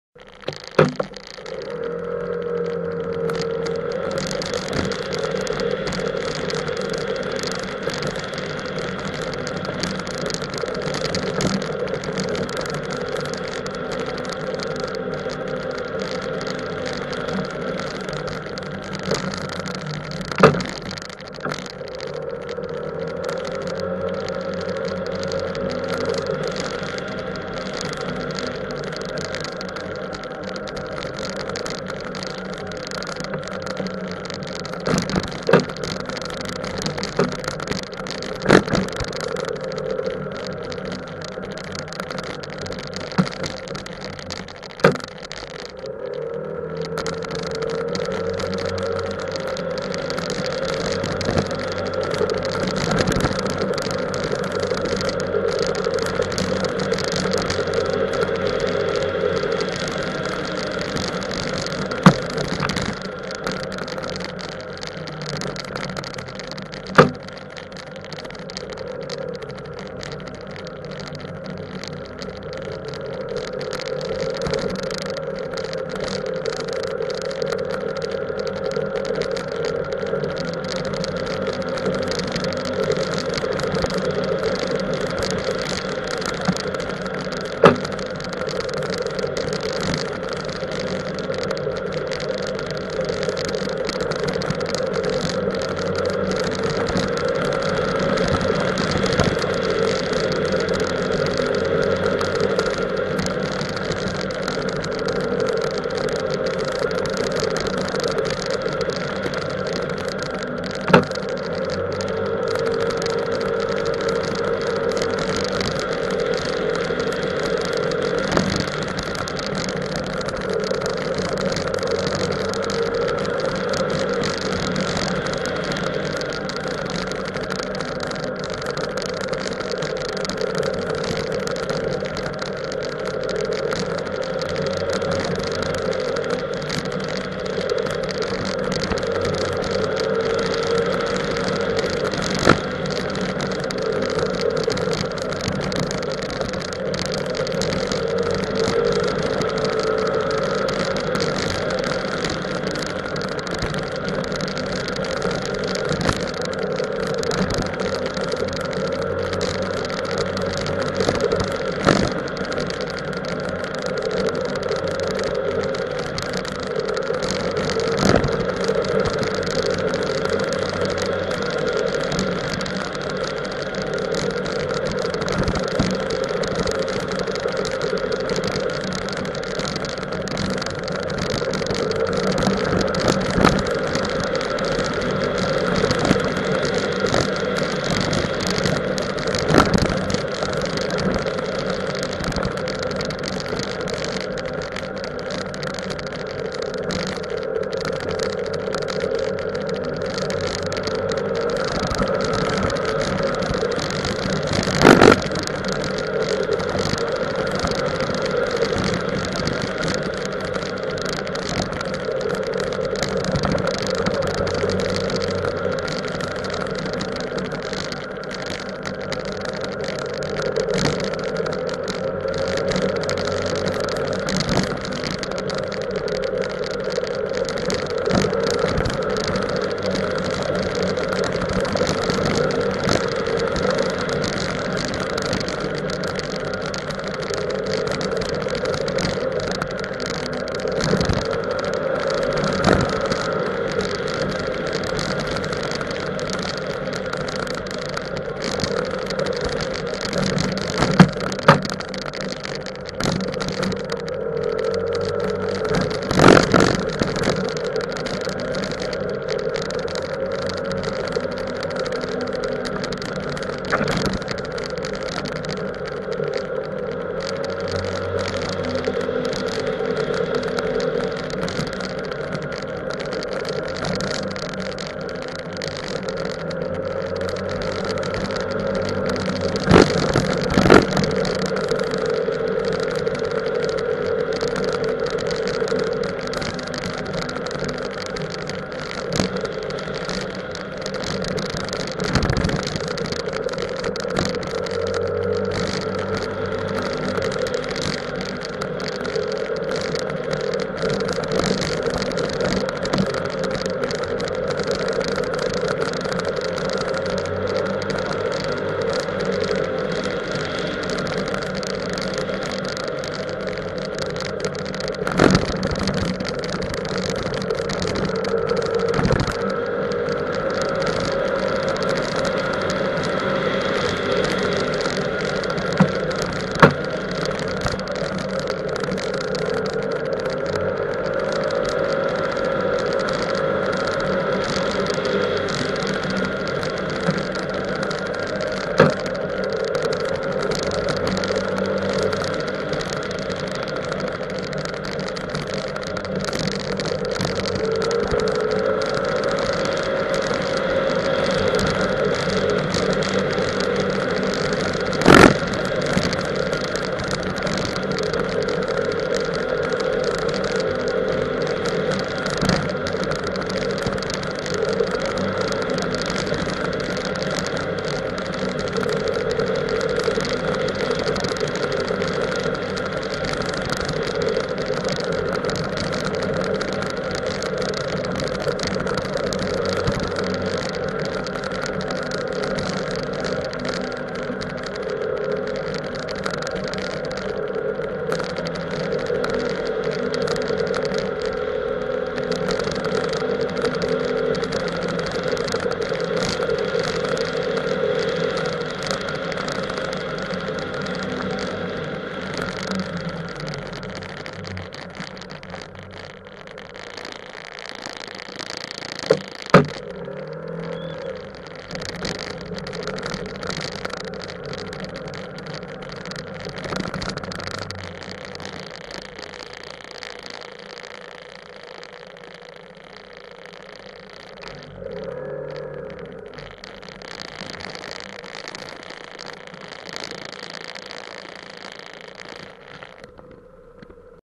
SCOOTERS EN BALADE - SUR LES ROUTES DE HAUTE-LOIRE - DIMANCHE 25 JUILLET 2021 (VIDÉO 4)
Ayant repris la Route des Gorges de la Loire à Vorey, je poursuis jusqu'à Lavoûte-sur-Loire et plutôt que de continuer je tournerai sur la D25 qui mène à Saint-Paulien, mais aussi à un panorama sur la région dans un petit Hameau qui domine les Sucs de Haute-Loire !